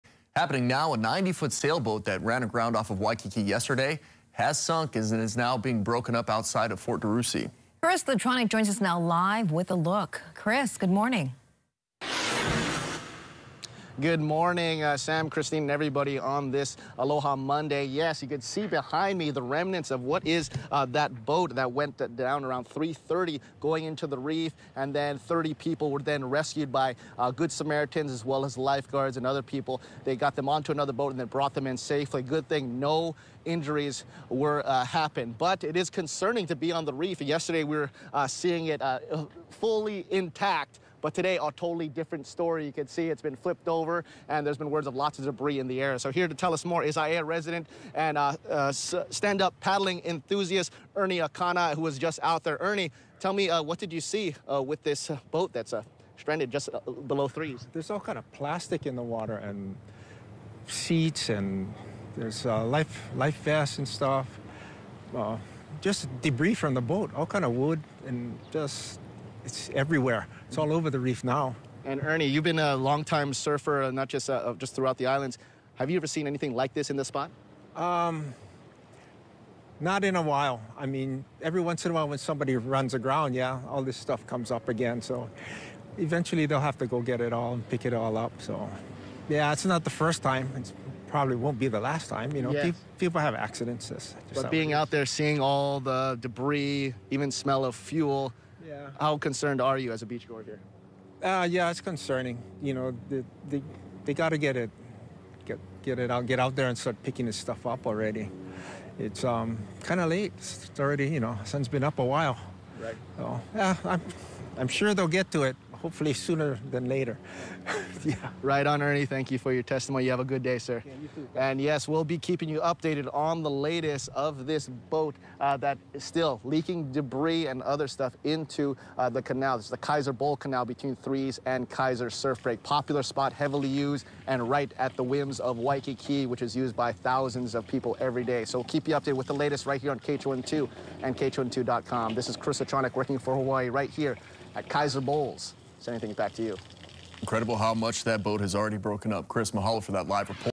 Sailboat that ran aground off Fort DeRussy has sunk
A 90-foot sailboat that ran aground on Sunday has sunk and is now breaking up outside of Fort DeRussy. We had a live look this morning.